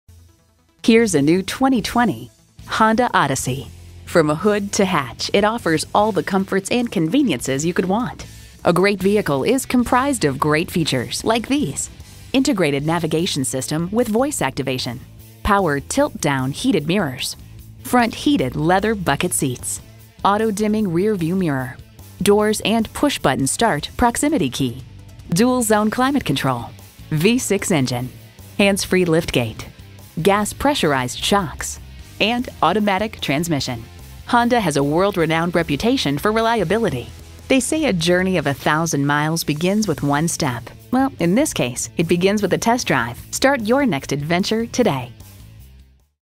New 2020 Honda Odyssey Elizabeth City, NC #420033 - SOLD
Year: 2020
Make: Honda
Model: Odyssey
Trim: Touring
Engine: 3.5L V6 SOHC i-VTEC 24V
Transmission: 10-Speed Automatic
Color: White
Interior: Brown
Stock #: 420033
VIN: 5FNRL6H8XLB018443

Address:
105 Tanglewood Parkway South